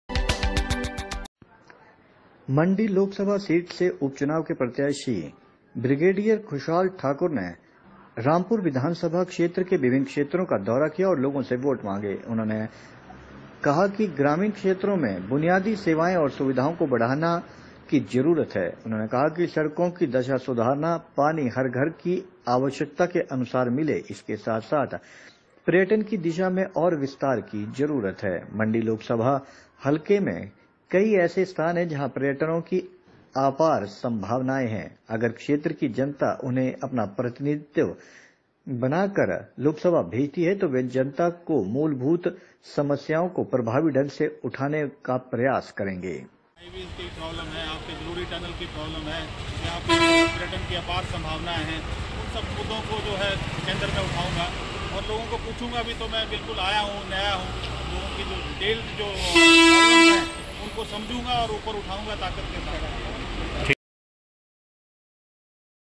Rampur भाजपा प्रत्याक्षीब्रिगेडियर कुशल ने रामपुर के विभिन्न क्षेत्रोंका दौरा कर लोगो से वोट मांगे
मंडी ससदीय हलके के उप चनाव में उतरे  भाजपा प्रत्याक्षी
ब्रिगेडियर कुशल ठाकुर ने आज रामपुर विधानसभा हल्के के विभिन्न क्षेत्रों
का दौरा कर लोगो से वोट मांगे।